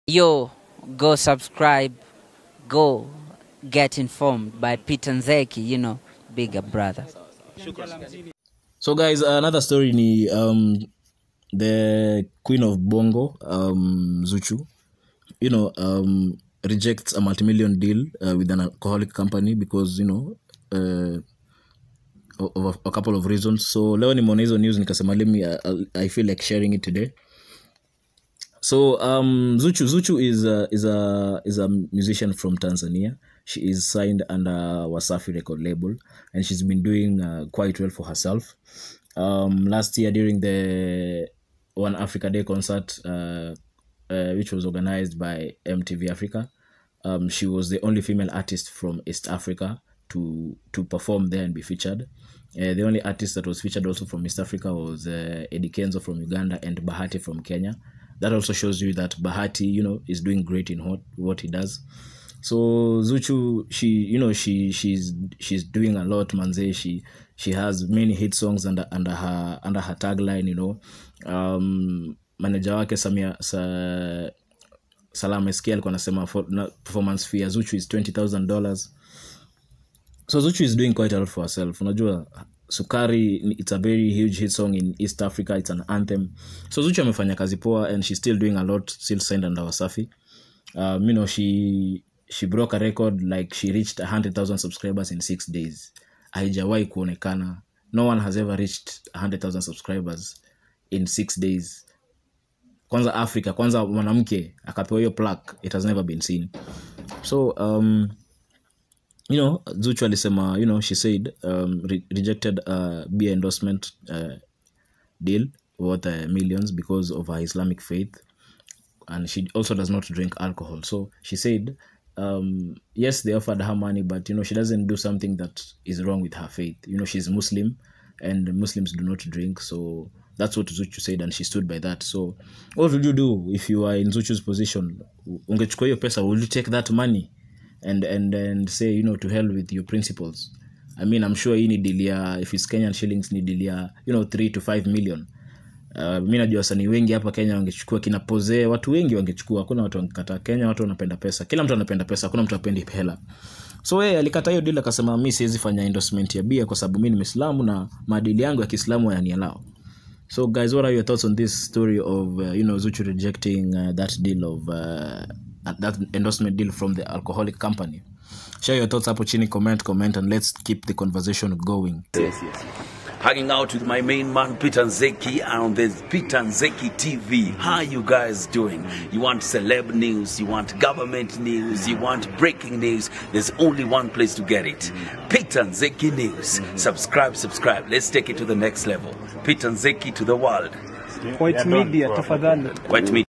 ZUCHU REJECTS MULTI-MILLION ENDORSEMENT FROM ALCOHOL COMPANY
tanzania queen of bongo Zuchu has revealed today that she rejected a MULTI-MILLION ENDORSEMENT from an alcohol company.